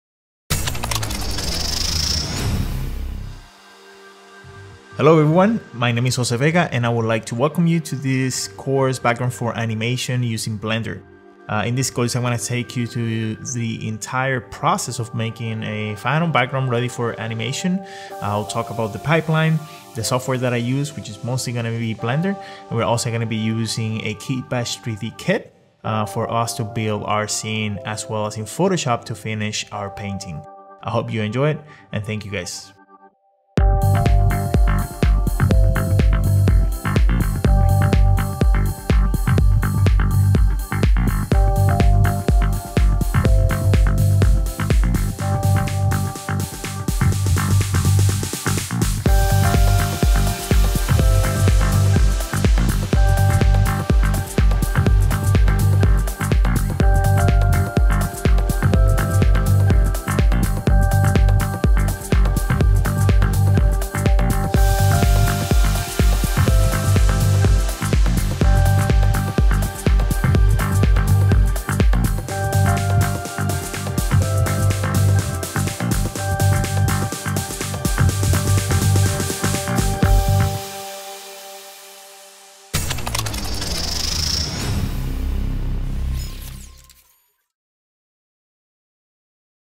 Designing Animation Backgrounds Using Blender & Photoshop With Jose Vega
Learn how to create final 2D background designs for animation with Jose Vega, former Background Design Supervisor on Netflix’s Castlevania

COMBINING KITBASHING & DIGITAL PAINTING TECHNIQUES

In this 4.5-hour workshop, Jose Vega demonstrates the speed at which 2D animation backgrounds can be created with the help of 3D assets, using Blender and Photoshop as his tools of choice. Starting with a rough storyboard, Jose demonstrates how to match a perspective and block out a scene in Blender before adding fine details and establishing the lighting with a combination of photo-bashing and digital painting in Photoshop.

Jose details the basics of Blender and covers how to incorporate 3D assets to quickly populate a background design with intricate details and powerful silhouettes. Using the “Neo City” Sci-Fi Mini Kit by KitBash3D, he teaches his techniques for isolating individual elements from larger models in Blender to create your own mini-assets to help populate and customize your scene even further.

Moving on to cover lighting and texturing in Blender, Jose proceeds to master a fully rendered scene complete with reflections, fog, and ambient occlusion, sharing his thought processes and techniques along the way. As the final step in his pipeline, he discusses his personal approach to using render passes generated from Blender to create alpha channels that aid the final polishing work done in Photoshop.

CHAPTERS:
01. Animation Backgrounds — Pipeline Breakdown
02. Introduction to Blender
03. Blocking Out a 3D Scene in Blender
04. Lighting Exploration in Blender
05. Color Thumbnails
06. Finishing the 3D Scene in Blender
07. Rendering Setup in Blender
08. Digital Painting in Photoshop
09. Creating Animation Backgrounds — Final Thoughts

For more workshops by industry masters that will help you take your creative skills to the next level, check out our complete catalog